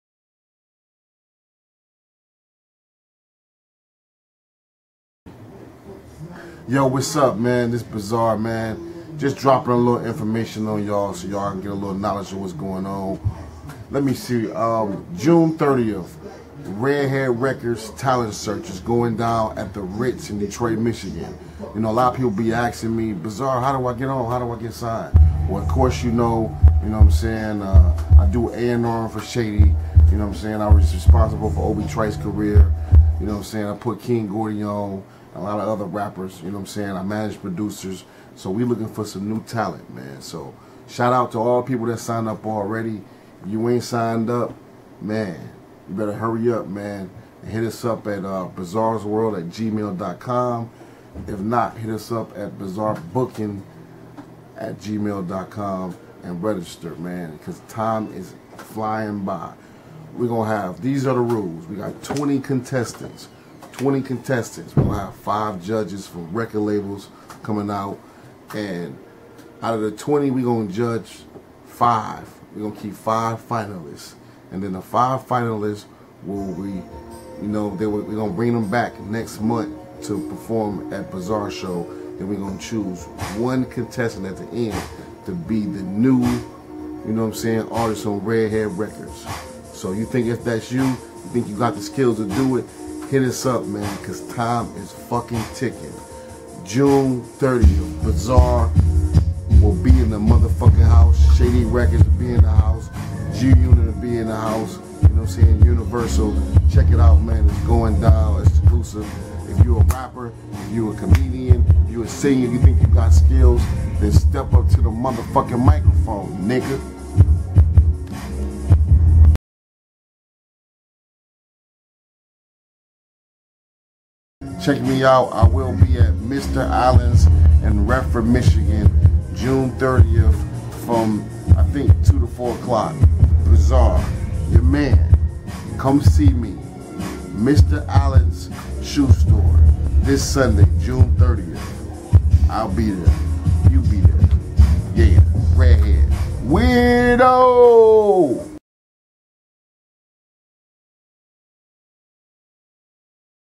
Bizarre's Talent Search and Instore Appearance 6/30/12
On 6/3/12

Come meet Bizarre at
Mr Alan's Shoes and Sportswear
14157 Telegraph Road
Redford Charter Township
2pm-4pm

and then come watch the talent search show with Bizarre and special guests at
The Ritz
24300 Hoover Rd, Warren, MI, 48089